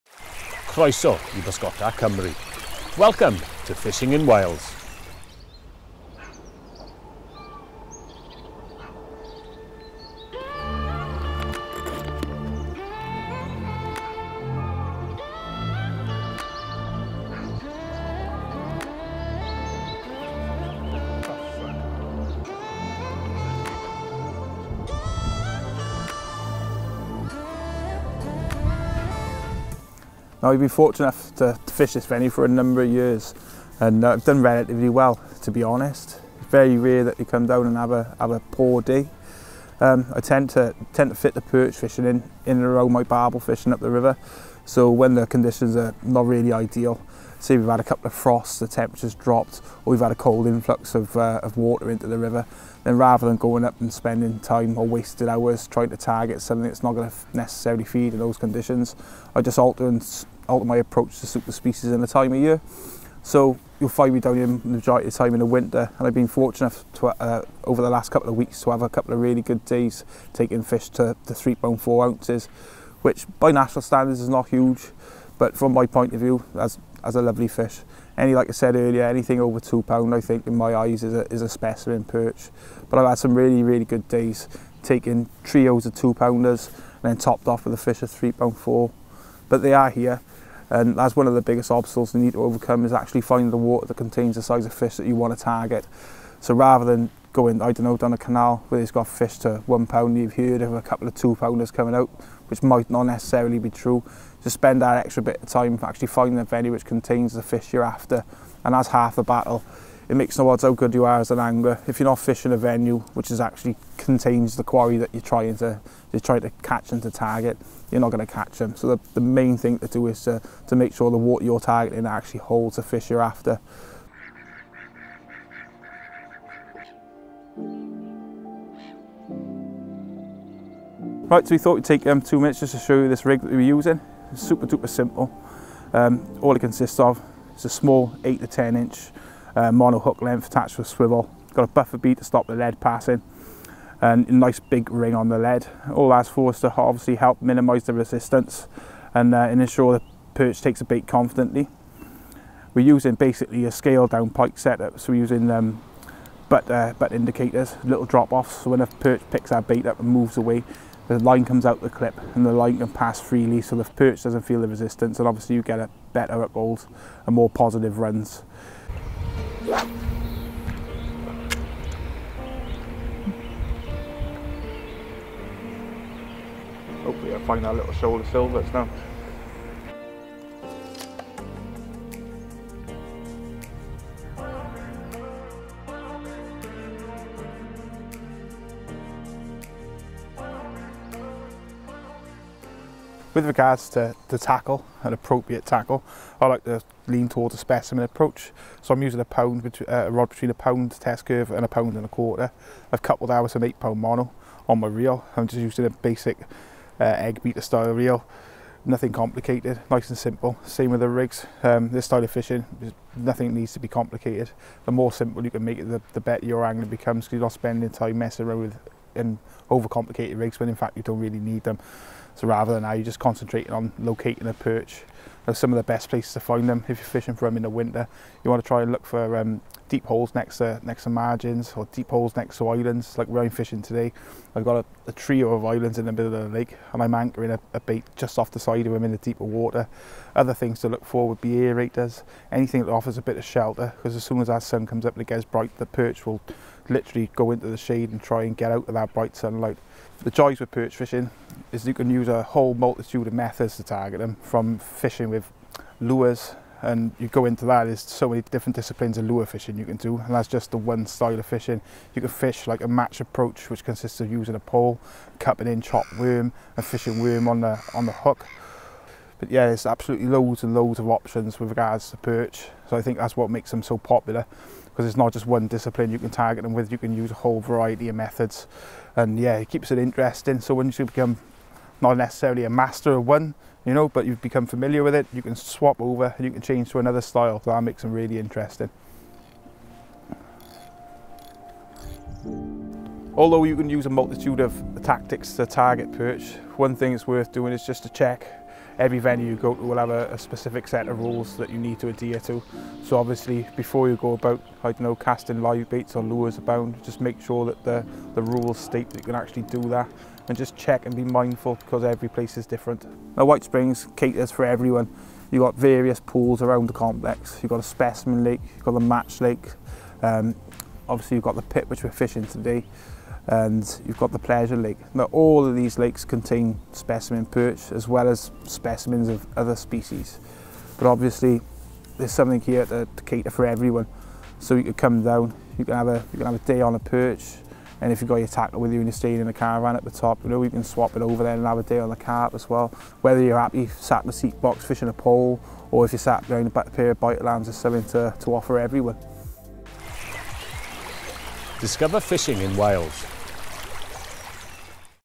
Perch Fishing at White Springs Fishery
Wales has fantastic specimen perch fishing opportunities if you know where to look! In this video we join Kevin Clark (Korum, The Angling Initiative) on the banks of White Springs Fishery near Swansea, where he shares his approach for big winter perch on commercial venues.